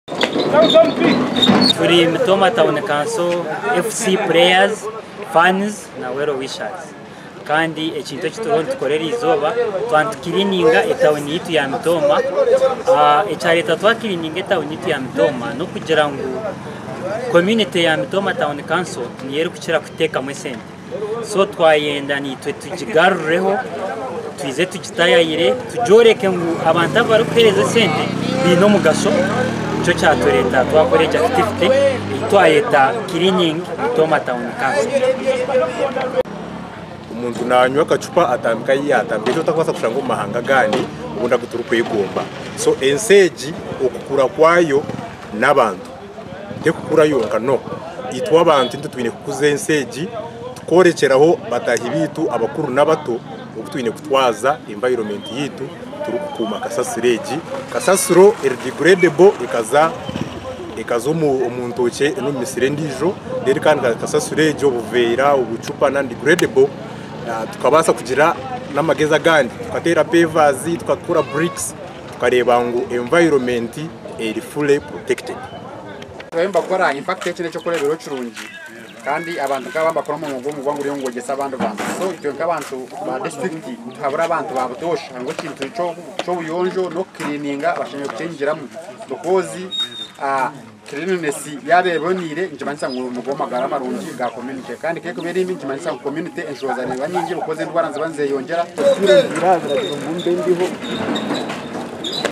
Agacencwire: Abazaani ba Mitooma FC bakozire burungi-bwa-nsi omukyanga kyabo
MITOOMA:

Abazaani ba Mitooma FC bakozire burungi-bwa-nsi omukyanga kyabo